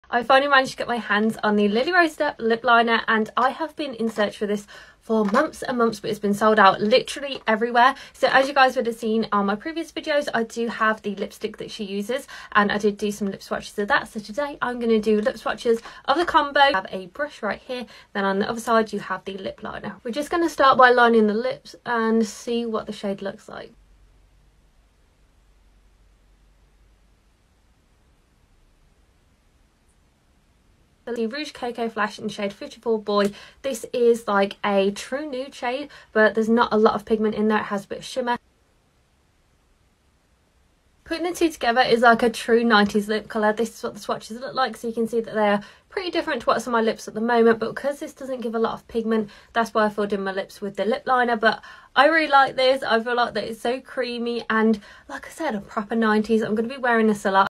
Testing Lily-Rose Depp’s Lip Combo - Lipliner & Lipstick!
Testing Lily-Rose Depp’s Lip Combo

Products used:
Chanel LE CRAYON LÈVRES Longwear Lip Pencil in 162 Nude Brun
Chanel Rouge Coco Flash in 54 Boy